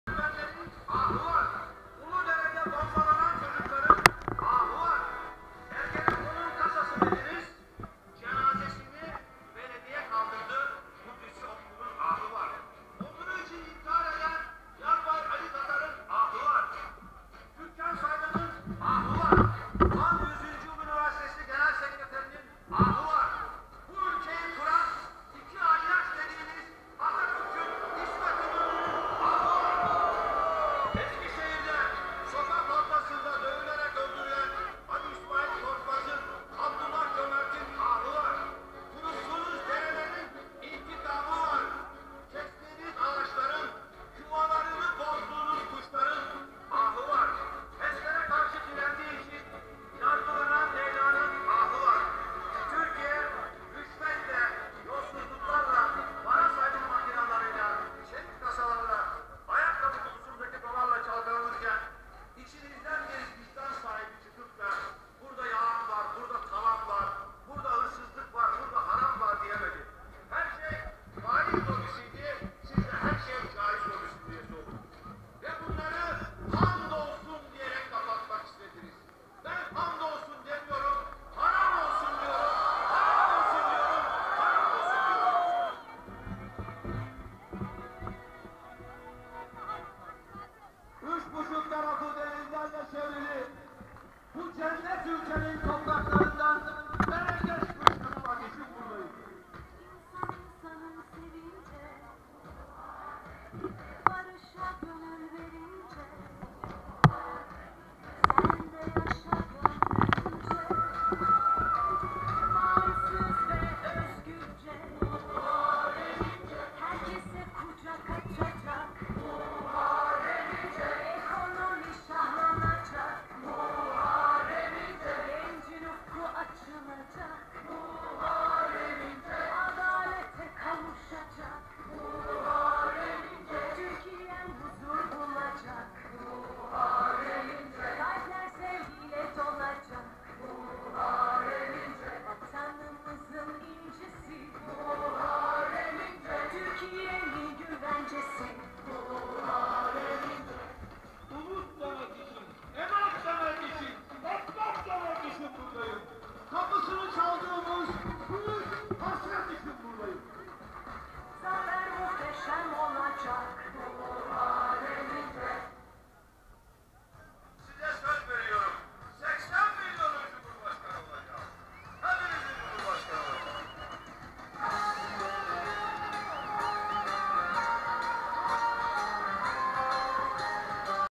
Acar tv 📺
Muharrem İnce'nin İstanbul Maltepe Mitingi 6 MİLYONLUK Şöleni